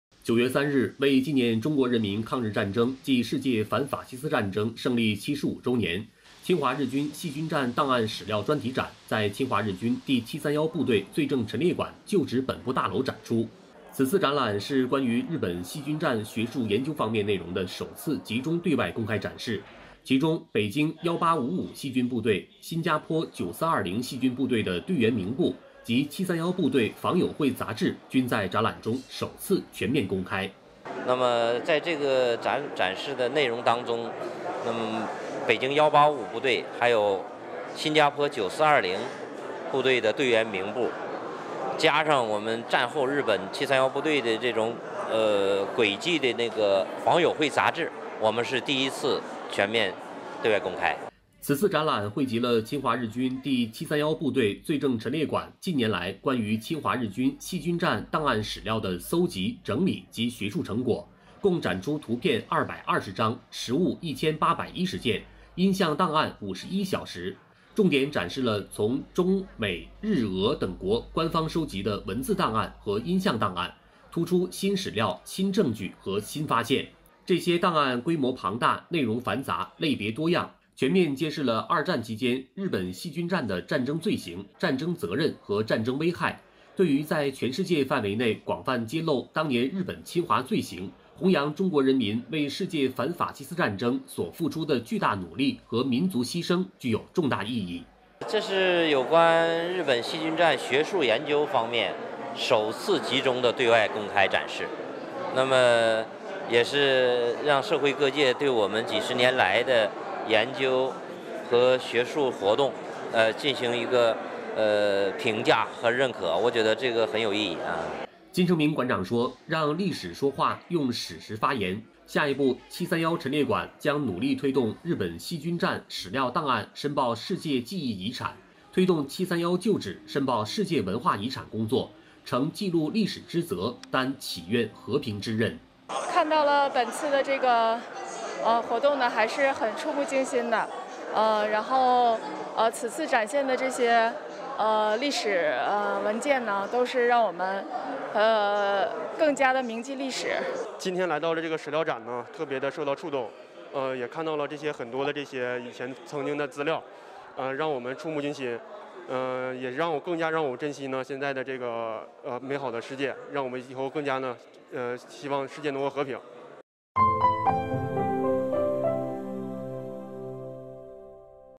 侵华日军细菌战档案史料展：两个细菌部队队员名簿首次公开
9 月 3 日，为纪念中国人民抗日战争暨世界反法西斯战争胜利 75 周年，" 侵华日军细菌战档案史料专题展 " 在侵华日军第七三一部队罪证陈列馆旧址本部大楼展出。此次展览是关于日本细菌战学术研究方面内容的首次集中对外公开展示，其中，北京一八五五细菌部队、新加坡九四二〇细菌部队的队员名簿及七三一部队房友会杂志均在展览中首次全面公开。